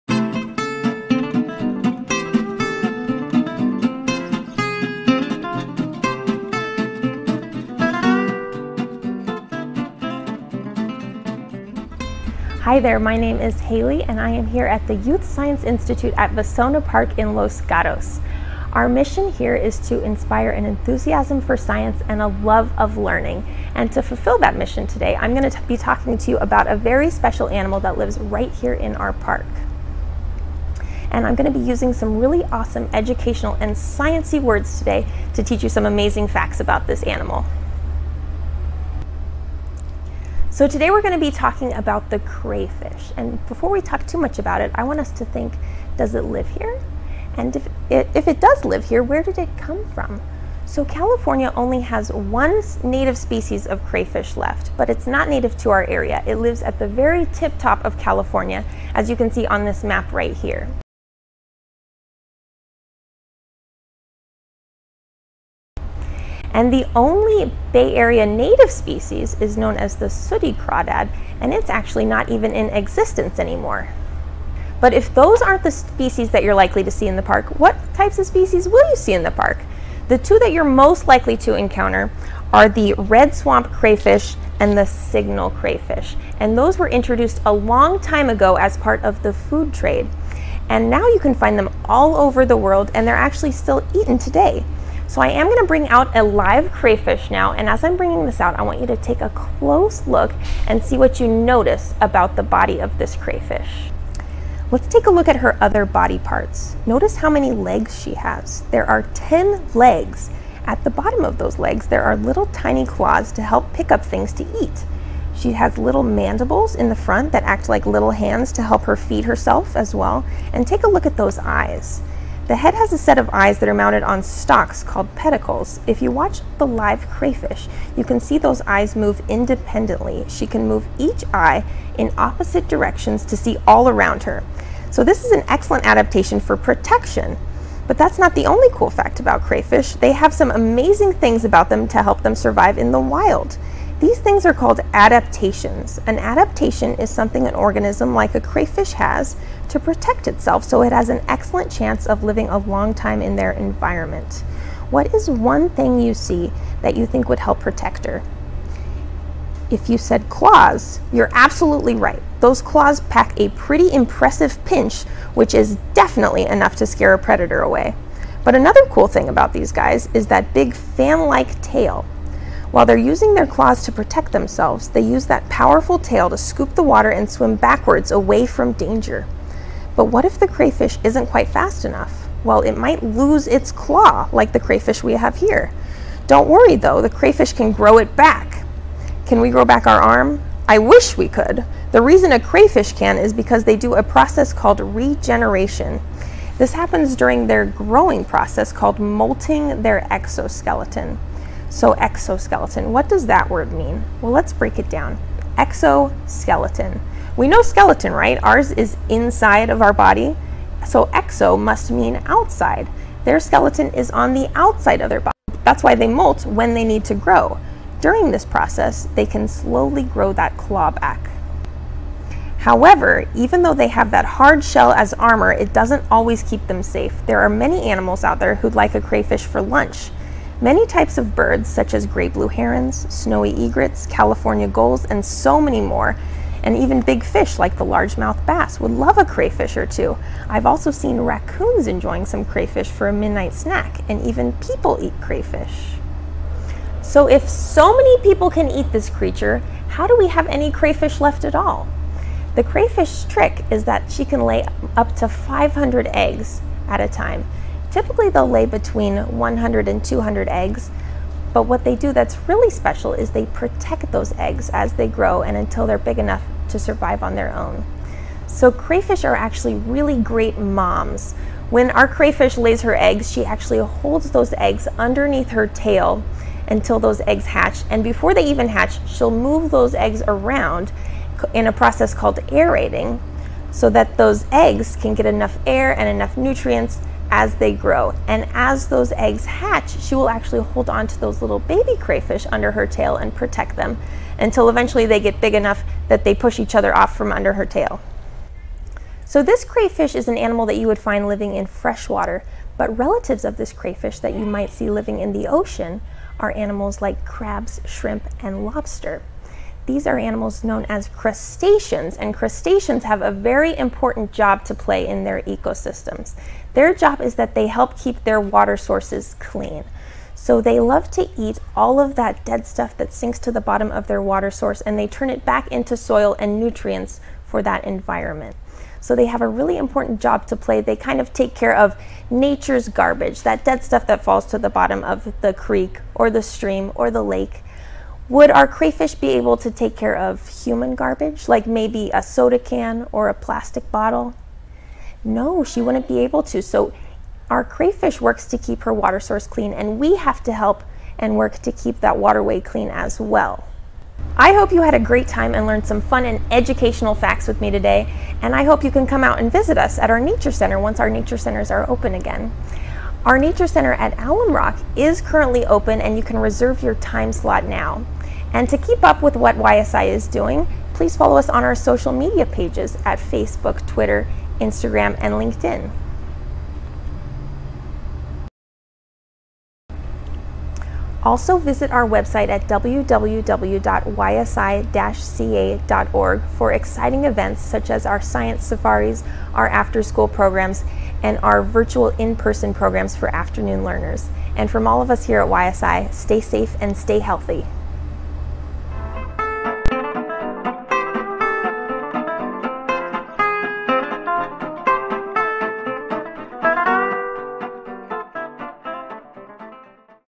WHD 2020: Youth Science Institute
Crayfish can be found in many of our local creeks, streams, and ponds here in Santa Clara County. The folks at Youth Science Institute are teaching us all sorts of cool facts about these awesome crustaceans.